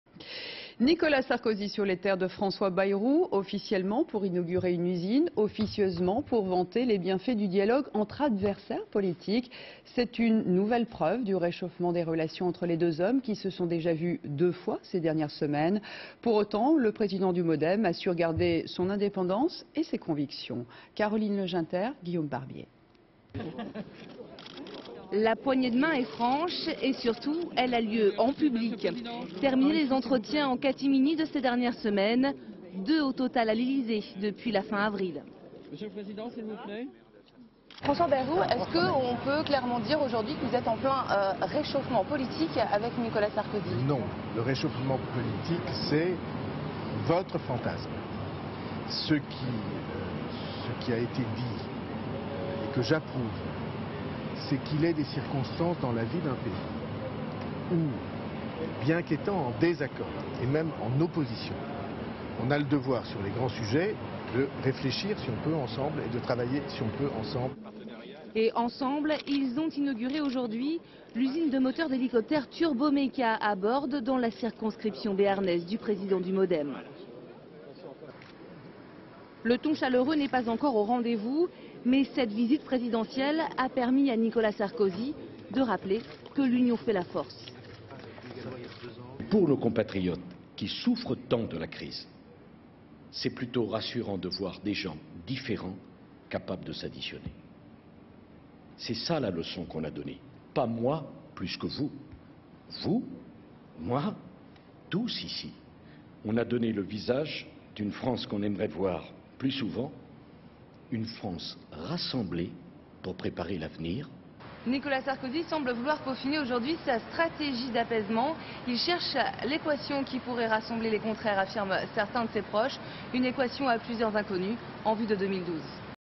Sarkozy et Bayrou visitent une usine à Bordes
22 juin 2010
[Source : documentation France 3]     Le président de la République a inauguré l'usine Turbomeca à Bordes, dans la circonscription béarnaise du président du MoDem, le troisième homme de l'élection de 2007.       François BAYROU, à la question de savoir s'il est en "réchauffement politique " avec Sarkozy: "Non. Le réchauffement politique, c'est votre fantasme. Ce qui a été dit et que j'approuve, c'est qu'il est des circonstances dans la vie d'un pays  où, bien quétant en désaccordet même en opposition, on a le devoir sur les grands sujets de réfléchir ensemble et de travailler si on peut ensemble".      Nicolas SARKOZY: "Pour nos compatriotes qui souffrent tant de la crise, c'est plutôt rassurant de voir des gens différents capables de s'additionner. C'est ça la leçon qu'on va donnéer. Pas moi plus que vous. Vous, moi, tous ici, on a donné le visage d'une France qu'on aimerait voir plus souvent. Une France rassemblée, pour préparer l'avenir". Images d'archive INA